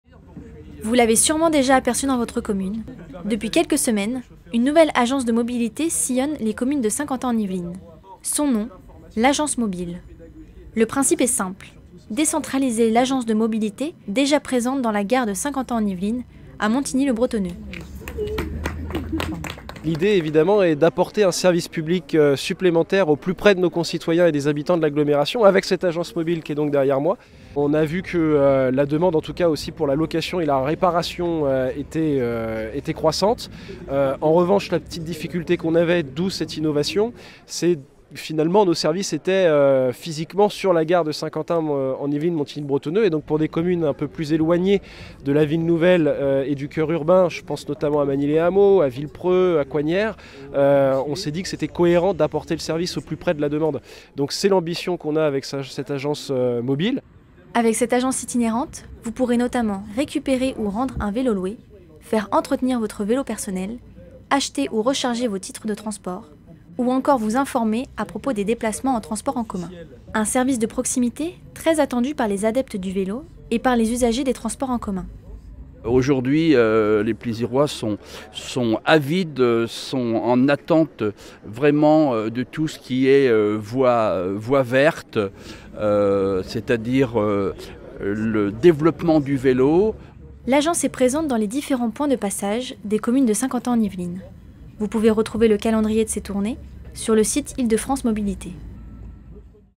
Saint-Quentin-en-Yvelines inaugure son agence mobile
Saint-Quentin-en-Yvelines inaugurait une nouvelle Vélostation, le 1e décembre, à Plaisir. Son nom : l’Agence Mobile. Cette version mobile de l’agence des mobilités ira à la rencontre des habitants dans les 12 communes de l’agglomération.